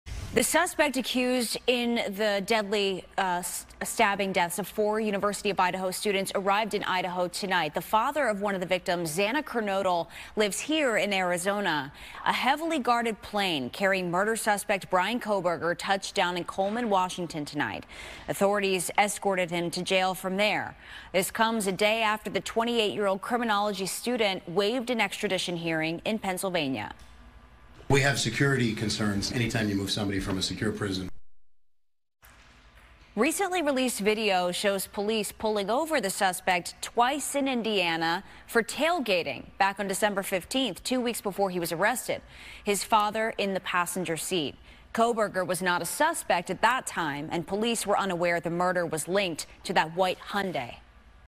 Murder suspect Bryan Kohberger arrives in Moscow, Idaho
The suspect accused in November's massacre of four University of Idaho students arrived in Idaho on the evening of Jan. 4. A heavily guarded plane carrying murder suspect Bryan Kohberger touched down in Coleman, Washington and authorities then escorted him to the Latah County Jail. This comes a day after the 28-year-old criminology student waived an extradition hearing in Pennsylvania.